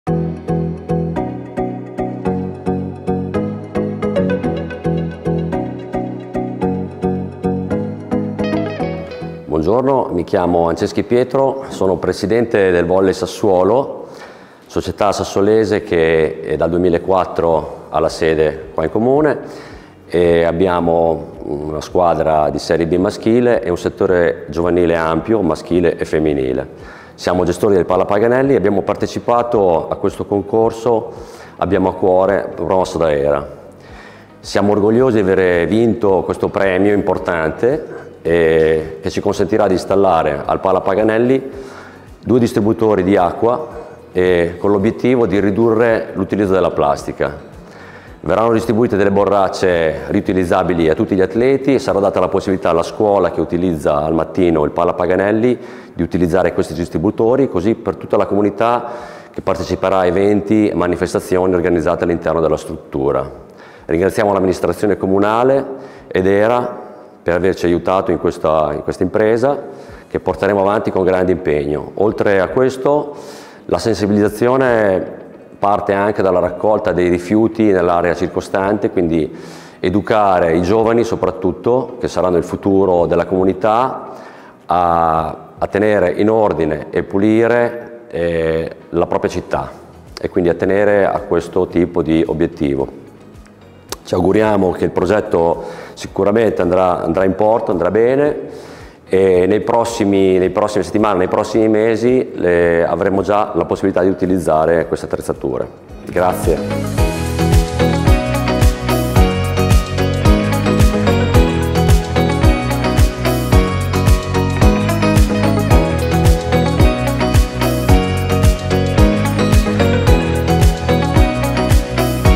HERA - Premiazione "Green Sport for Community", a Sassuolo la pallavolo è sostenibile
Il primo premio in strumenti e attrezzature è stato assegnato all’Associazione Sportiva Dilettantistica ‘Volley Sassuolo’ la cui proposta prevede la riduzione della produzione di rifiuti da imballaggi in plastica monouso grazie all’installazione di erogatori di acqua di rete all’interno del Palapaganelli, la promozione della raccolta differenziata e l’avvio di una campagna di sensibilizzazione del decoro urbano tramite attività bisettimanali di plogging, svolte dalle squadre giovanili della Volley Sassuolo Asd.